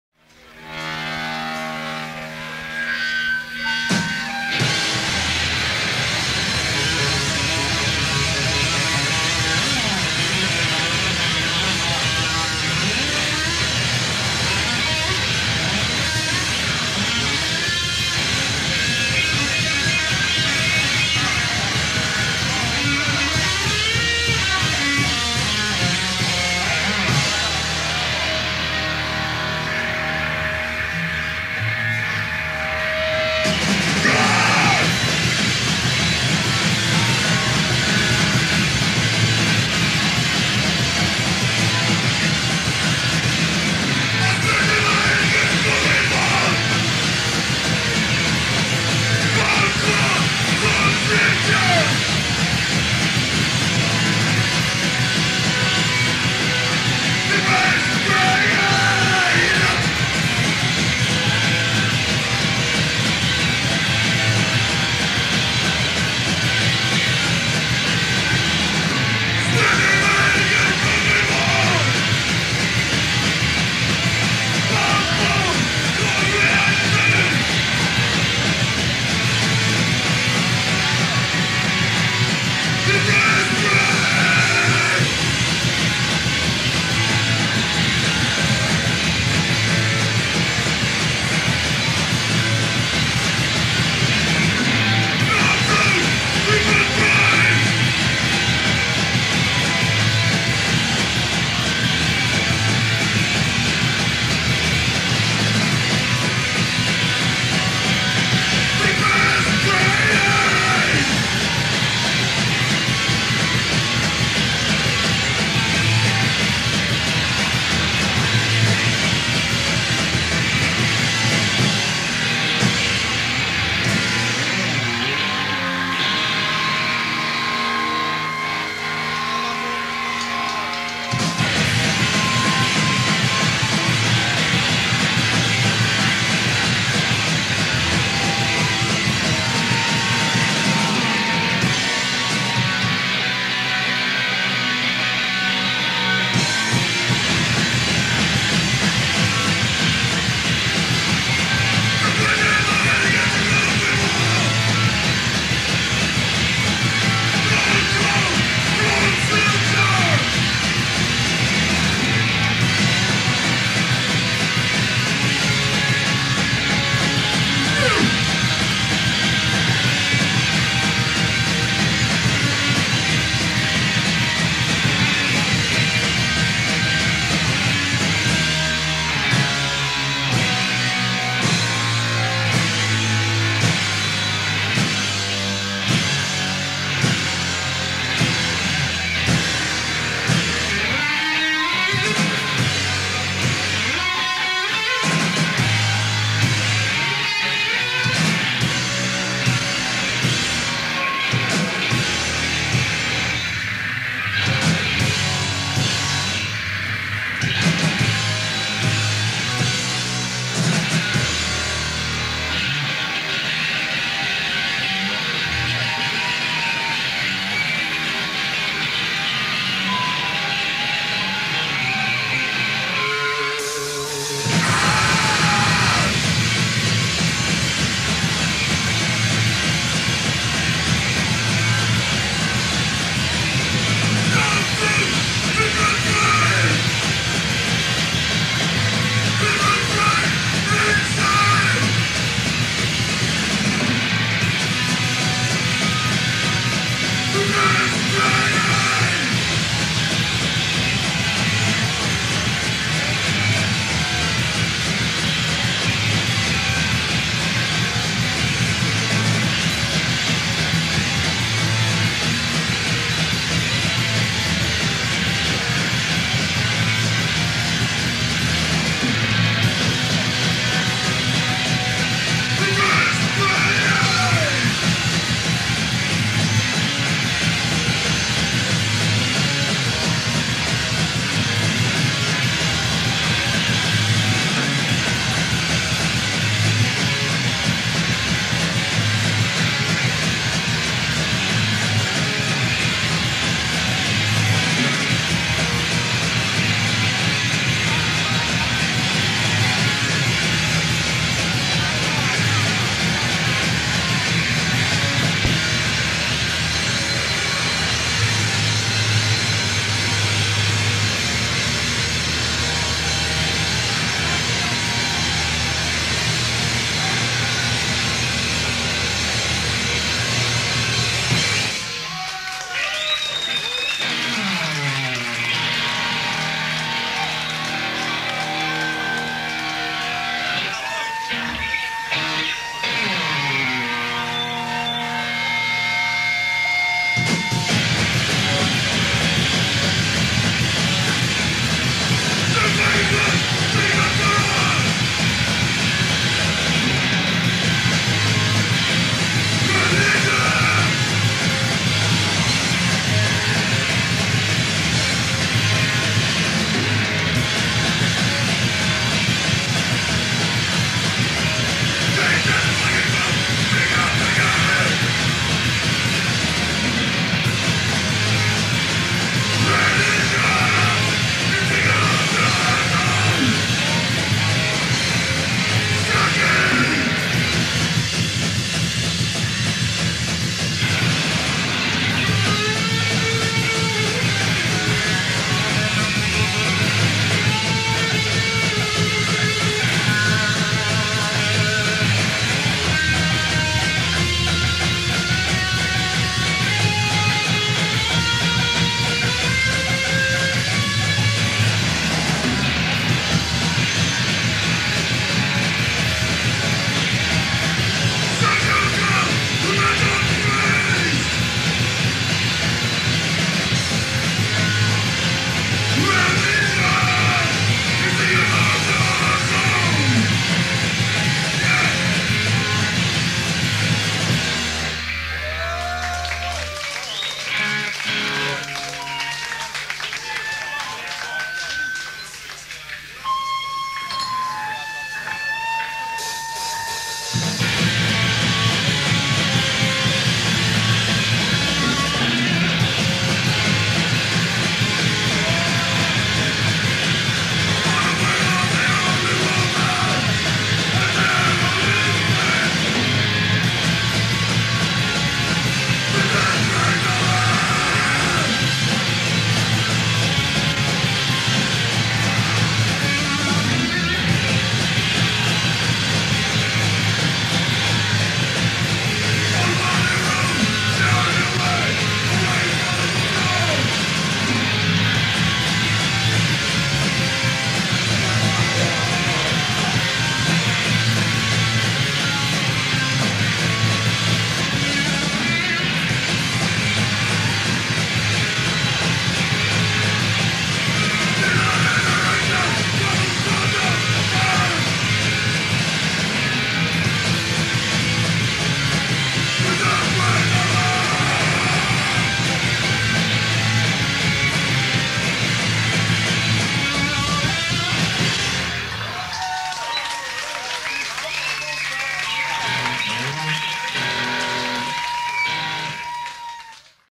ANTI-CIMEX - April 1985 No Location Info [AUDIENCE AUDIO]
Band: Anti-Cimex
Date: April 1985
Location: unknown
Venue: unknown
Source:  AUD
Lineage: ANA(x)- WAV - FLAC
Equipment: Nakamichi CR-7A - FocusRite Scarlett 816 - Audio Hijack - FCPX
Recorded by: unknown
Setlist:
01.
02.
03.
04.
05.
Additional Info:
Incomplete.  Volume +6 db
Transferred by DH